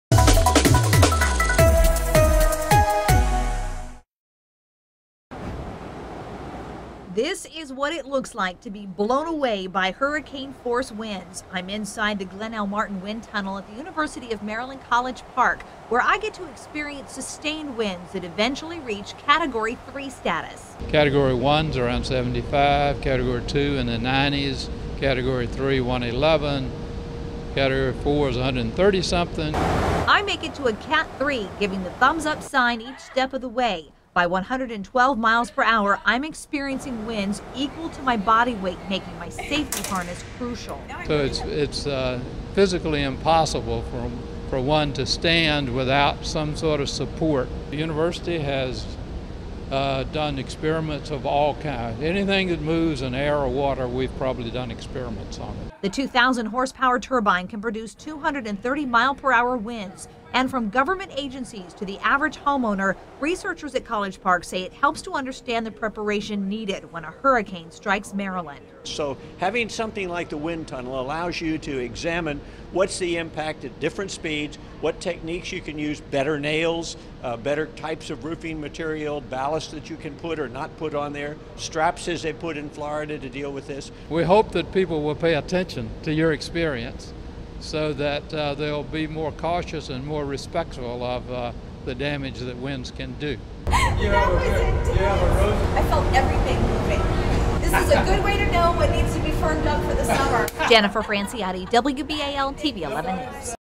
Wind tunnel research helps shore up against hurricanes
Hurricane season is here, with NOAA predicting a near-normal season, meaning there's a 70 percent chance for four to eight named storms. Disaster officials say preparation is key.